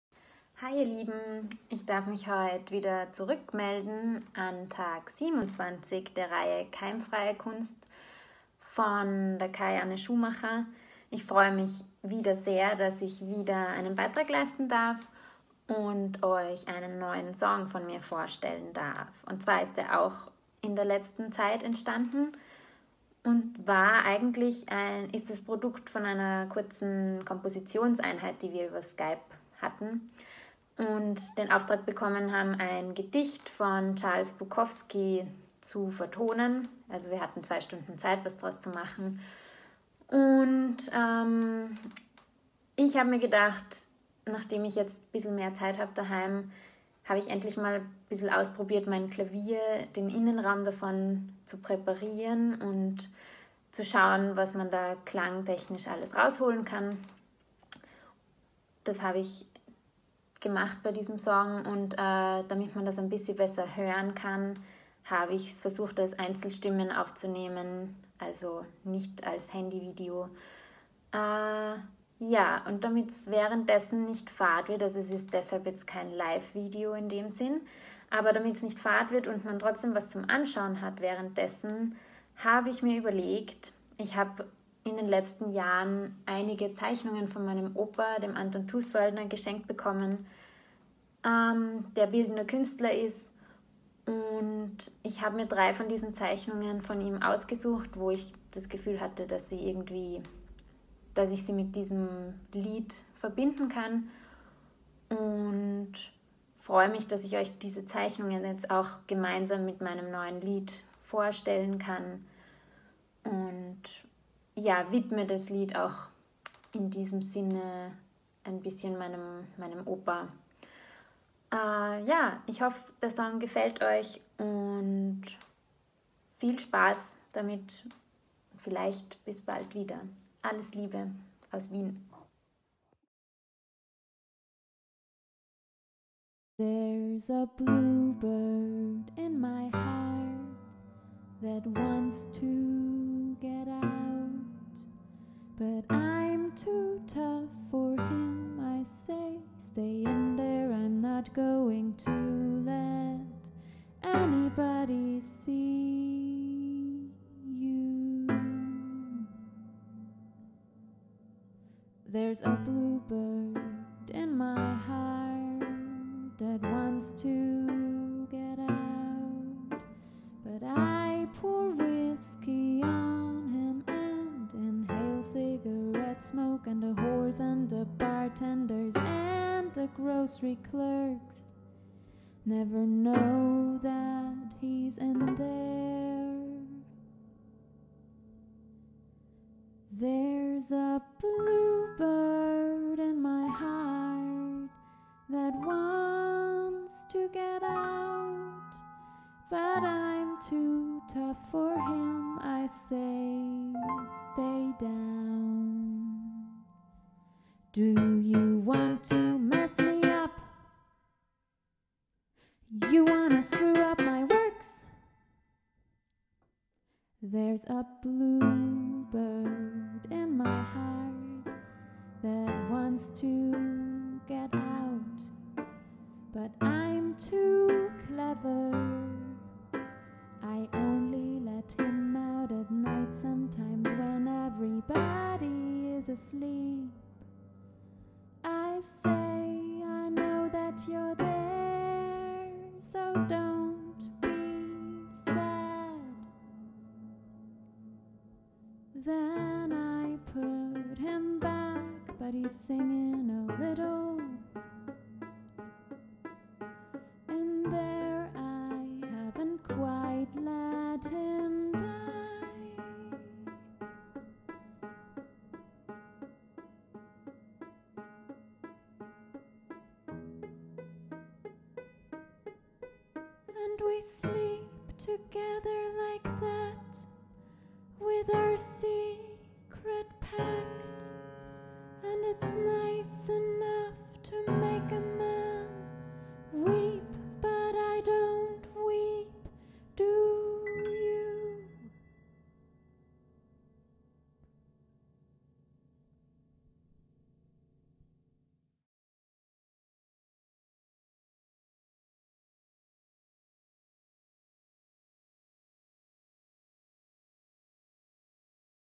Keimfreie Kunst, Tag 27
Heute ist wieder Pianistin und Komponisitin Anna Defant bei uns. Die Quarantäne scheint kreativ zu machen, denn schon wieder gibt es einen nigelnagelneuen Song. Diesmal mit besonderem Extra: Anna hat ein paar Gemälde ihres Großvaters, des Bildnenden Künstlers Anton Thuswaldner zu Betrachtung auf die Video-Spur gelegt. Wirklich ein besonderer Mix. Vielen Dank!